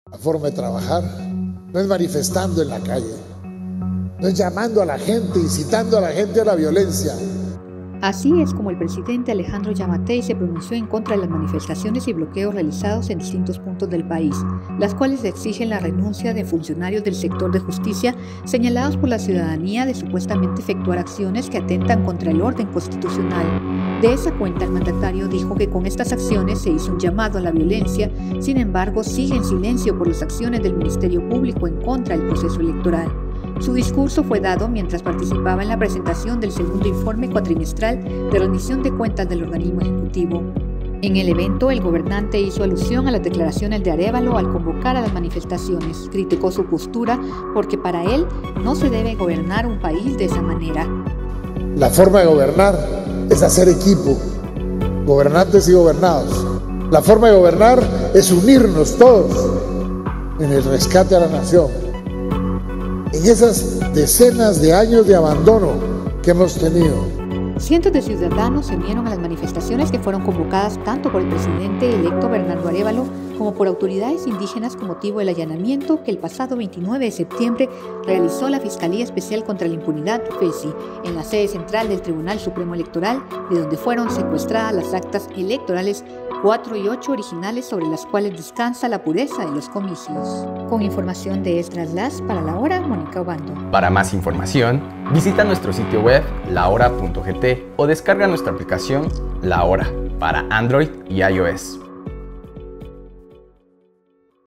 Giammattei sigue en silencio por acciones del MP
Giammattei se pronunció ante las manifestaciones  realizadas a nivel nacional, donde exigen la renuncia de funcionarios del sector de la justicia y del Ministerio Público.  Aquí detalles👇🏻